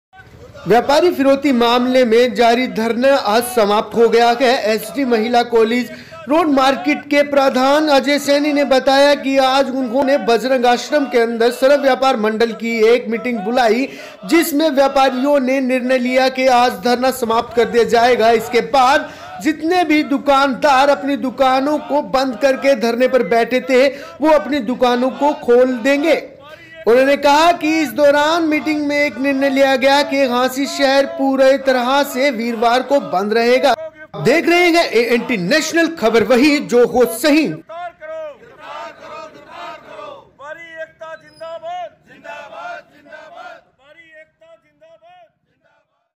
फिरौती मामले में चल रहा धरना समाप्त,वीरवार को हांसी बन्द करने का सर्व व्यापार मंडल ने लिया निर्णय
"ANT national"ख़बर वही जो हो सही,हांसी : फिरौती मामले में चल रहा धरना समाप्त,वीरवार को हांसी बन्द करने का सर्व व्यापार मंडल ने लिया निर्णय